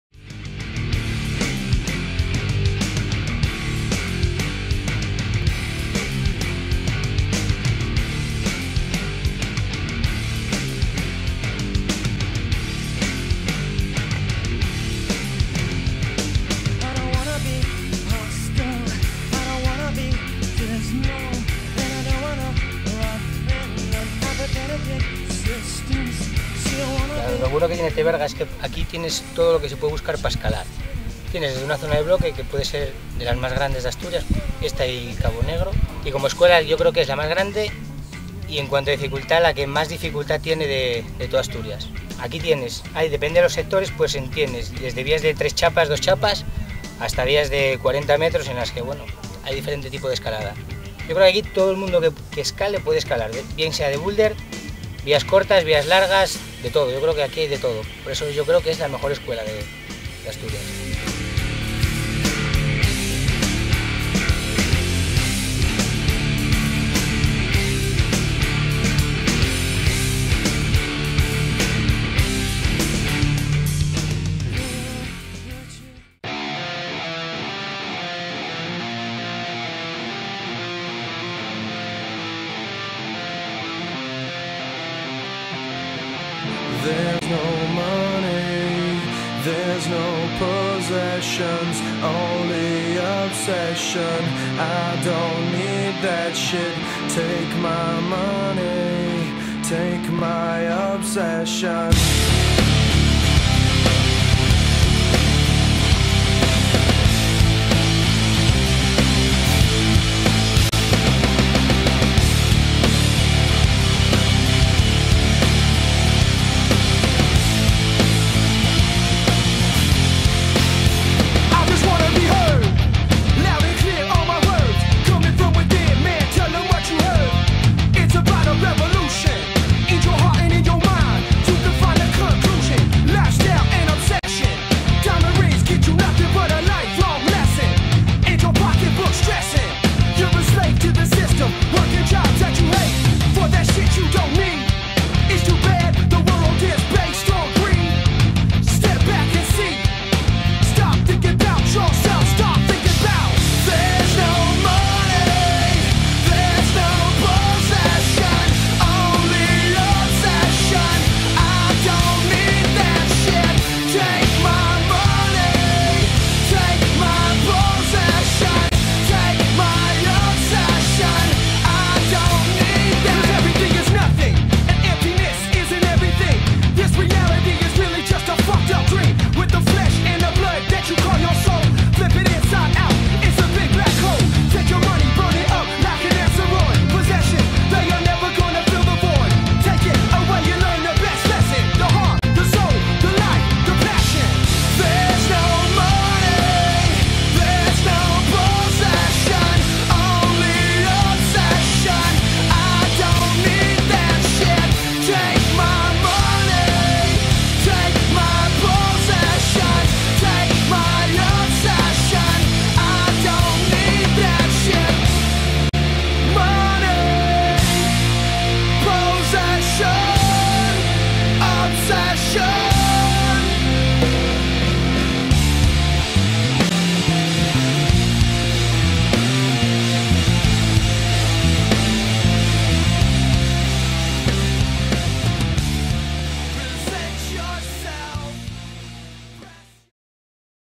teverga-bloques.mov
video de escalada en bloques (bulder) en la escuela de Teverga, Asturias
Tino Blanco y Jairo Pandiella